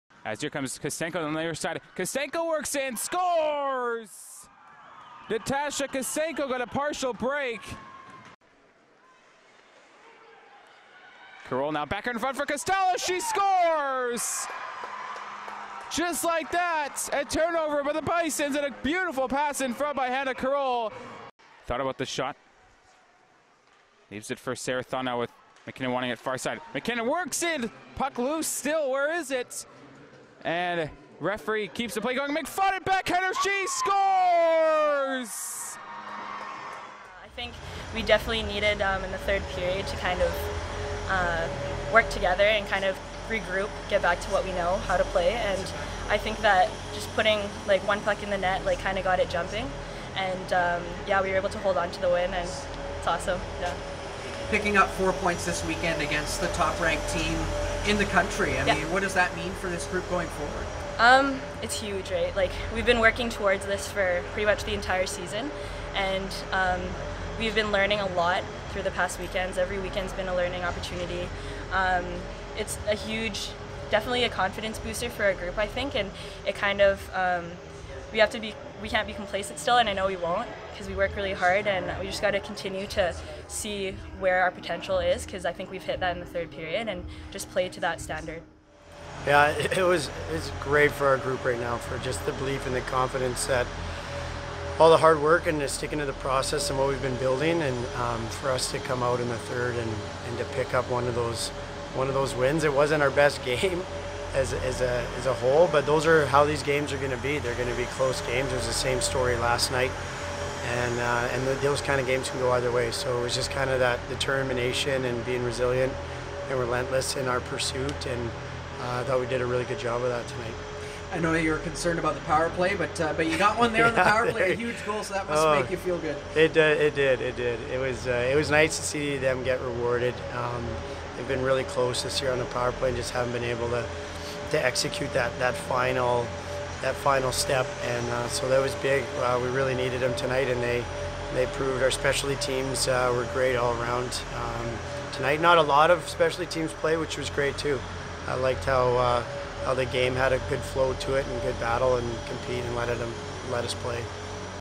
UBC 2, Manitoba 1 - November 17, 2018
The UBC Thunderbirds scored twice in the third period to earn a 2-1 win over the No. 1 ranked Manitoba Bisons